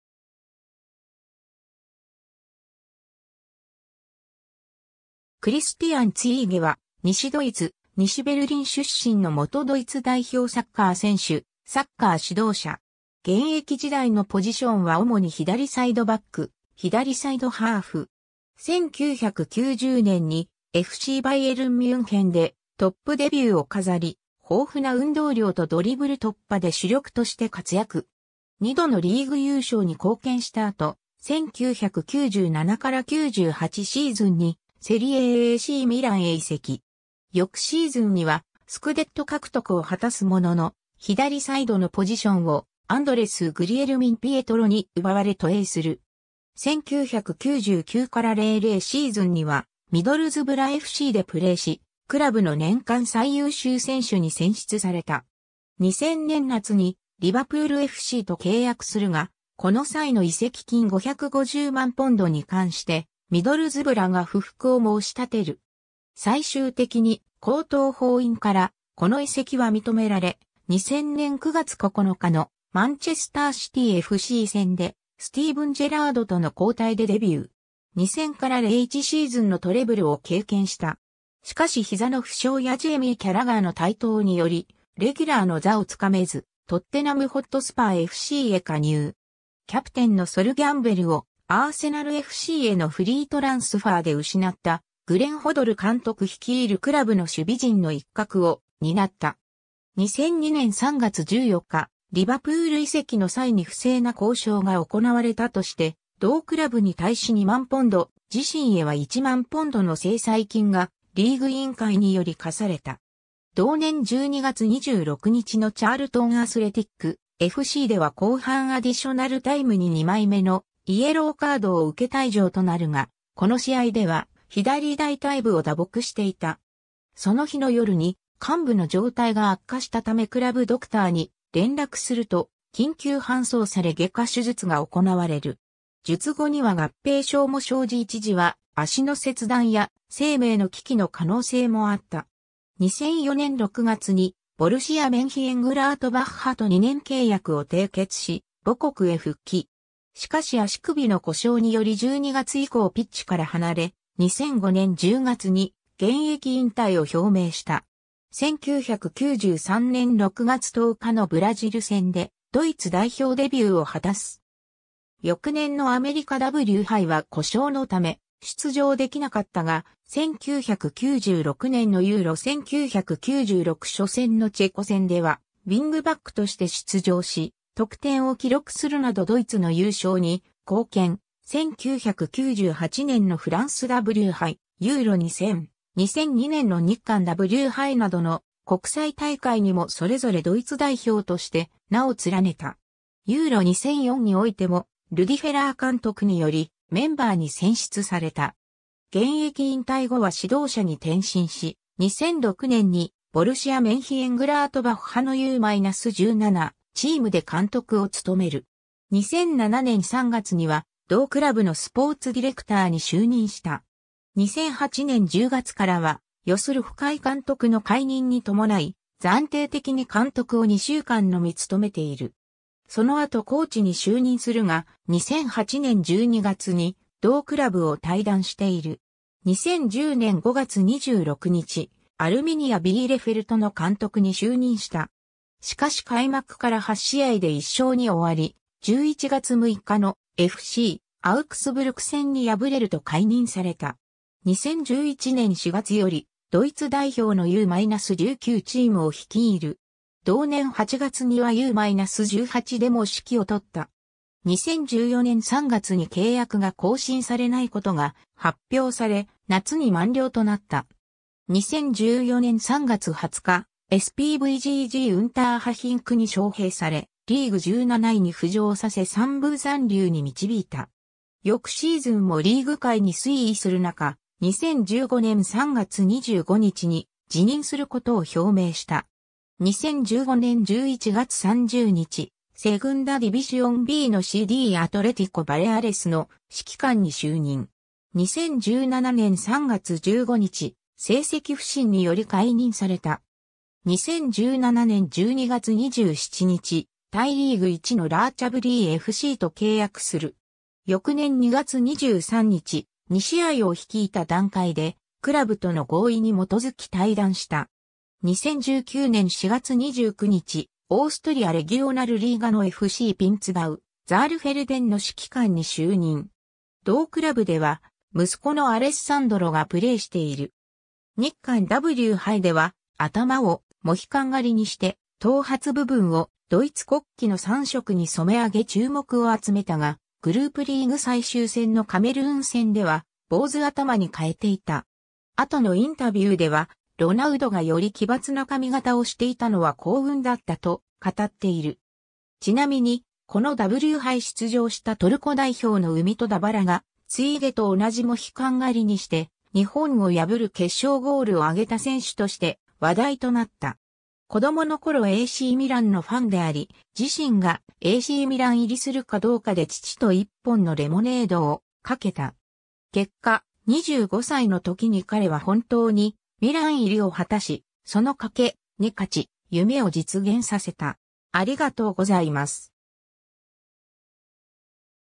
クリスティアン・ツィーゲ
クリスティアン・ツィーゲ（Christian Ziege, 1972年2月1日 - ）は、西ドイツ・西ベルリン出身の元ドイツ代表サッカー選手、サッカー指導者。
現役時代のポジションは主に左サイドバック、左サイドハーフ。
 1990年にFCバイエルン・ミュンヘンでトップデビューを飾り、豊富な運動量とドリブル突破で主力として活躍。
2度のリーグ優勝に貢献した後、1997-98シーズンにセリエA・ACミランへ移籍。
翌シーズンにはスクデット獲得を果たすものの、左サイドのポジションをアンドレス・グリエルミンピエトロに奪われ渡英する。
 1999-00シーズンにはミドルズブラFCでプレーし、クラブの年間最優秀選手に選出された。
2000年夏にリヴァプールFCと契約するが、この際の移籍金550万ポンドに関してミドルズブラが不服を申し立てる。
最終的に高等法院からこの移籍は認められ、2000年9月9日のマンチェスター・シティFC戦でスティーヴン・ジェラードとの交代でデビュー。
2000-01シーズンのトレブルを経験した。
 しかし膝の負傷やジェイミー・キャラガーの台頭によりレギュラーの座を掴めず、トッテナム・ホットスパーFCへ加入。
キャプテンのソル・キャンベルをアーセナルFCへのフリートランスファーで失ったグレン・ホドル監督率いるクラブの守備陣の一角を担った。
 2002年3月14日、リヴァプール移籍の際に不正な交渉が行われたとして、同クラブに対し2万ポンド、自身へは1万ポンドの制裁金がリーグ委員会により科された。
同年12月26日のチャールトン・アスレティックFCでは後半アディショナルタイムに2枚目のイエローカードを受け退場となるが、この試合では左大腿部を打撲していた。
その日の夜に患部の状態が悪化したためクラブドクターに連絡すると、緊急搬送され外科手術が行われる。
術後には合併症も生じ一時は足の切断や生命の危機の可能性もあった。
 2004年6月にボルシア・メンヒェングラートバッハと2年契約を締結し母国へ復帰。
しかし足首の故障により12月以降ピッチから離れ、2005年10月に現役引退を表明した。
 1993年6月10日のブラジル戦でドイツ代表デビューを果たす。
翌年のアメリカW杯は故障のため出場できなかったが、1996年のEURO1996初戦のチェコ戦ではウィングバックとして出場し得点を記録するなどドイツの優勝に貢献、1998年のフランスW杯、EURO2000、2002年の日韓W杯などの国際大会にもそれぞれドイツ代表として名を連ねた。
EURO2004においてもルディ・フェラー監督によりメンバーに選出された。
 現役引退後は指導者に転身し、2006年にボルシア・メンヒェングラートバッハのU-17チームで監督を務める。
2007年3月には同クラブのスポーツディレクターに就任した。
2008年10月からはヨス・ルフカイ監督の解任に伴い、暫定的に監督を2週間のみ務めている。
その後コーチに就任するが、2008年12月に同クラブを退団している。
 2010年5月26日、アルミニア・ビーレフェルトの監督に就任した。
しかし開幕から8試合で1勝に終わり、11月6日のFCアウクスブルク戦に敗れると解任された。
 2011年4月よりドイツ代表のU-19チームを率いる。
同年8月にはU-18でも指揮を執った。
2014年3月に契約が更新されないことが発表され、夏に満了となった。
 2014年3月20日、SpVggウンターハヒンクに招聘され、リーグ17位に浮上させ3部残留に導いた。
翌シーズンもリーグ下位に推移する中、2015年3月25日に辞任することを表明した。
 2015年11月30日、セグンダ・ディビシオンBのCDアトレティコ・バレアレスの指揮官に就任。
2017年3月15日、成績不振により解任された。
 2017年12月27日、タイ・リーグ1のラーチャブリーFCと契約する。
翌年2月23日、2試合を率いた段階でクラブとの合意に基づき退団した。
 2019年4月29日、オーストリア・レギオナルリーガ（3部相当）のFCピンツガウ・ザールフェルデンの指揮官に就任。
同クラブでは息子のアレッサンドロがプレ...